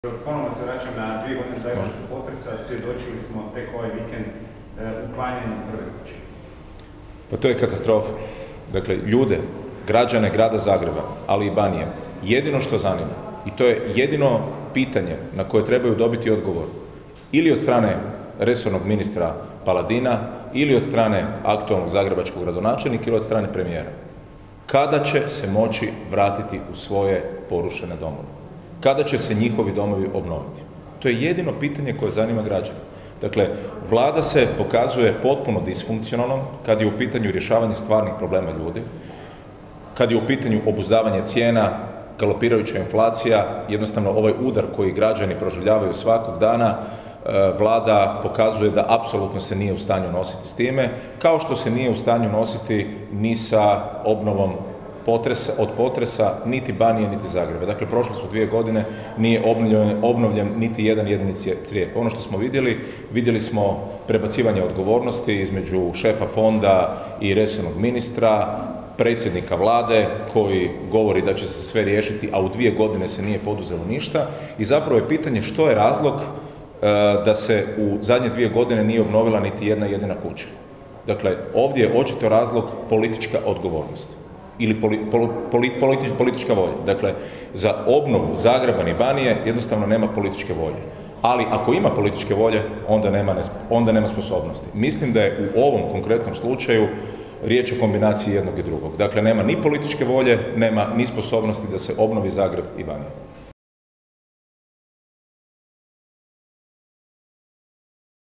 Davor Bernadić o obnovi Zagreba i Banovine
Zastupnik Davor Bernardić na konferenciji za medije održanoj u Saboru govorio je o obnovi Zagreba i Banovine.